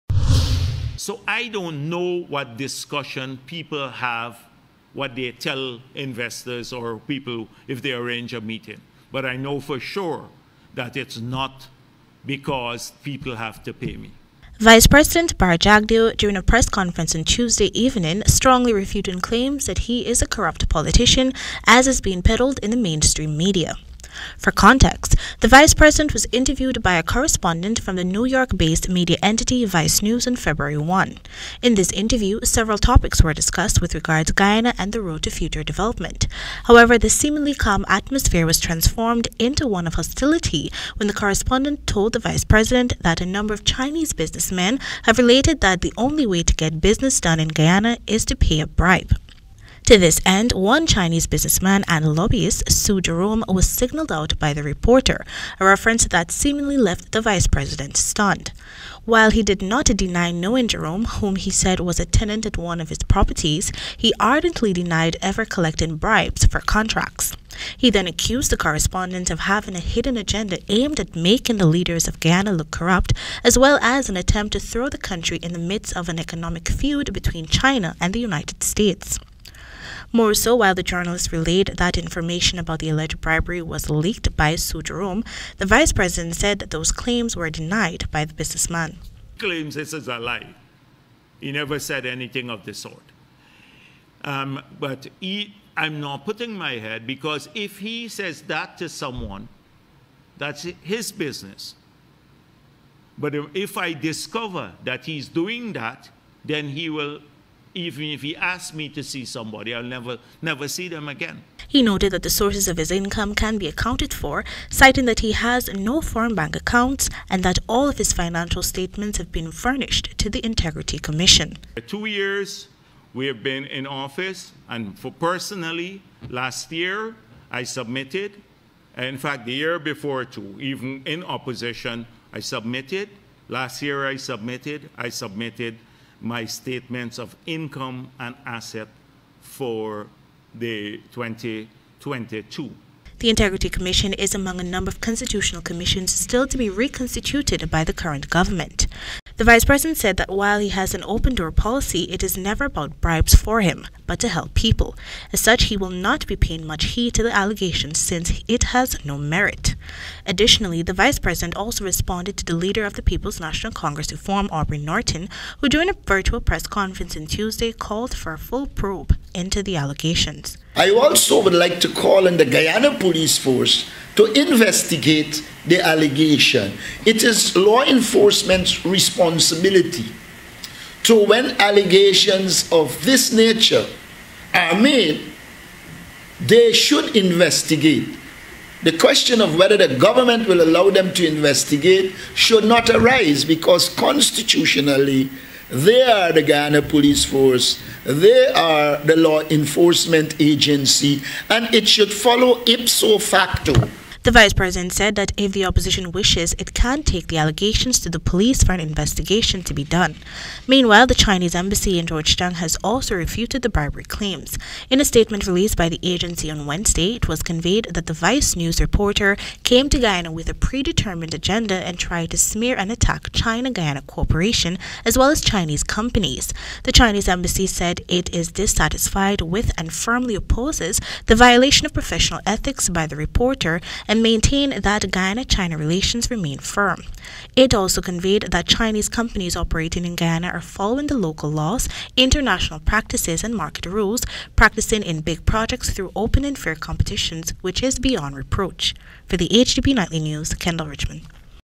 MORE DENIAL FROM VICE PRESIDENT BHARRAT IN BRIBERY SCANDAL
With a massive bribery allegation hanging over his head, Vice President Bharrat Jagdeo continues to deny claims that he is a venal politician. The Vice President has classified the questions of a Vice News reporter regarding bribery claims as a ploy to drag Guyana into the economic wrestle between the United States and China. Kendell Richmond tells us more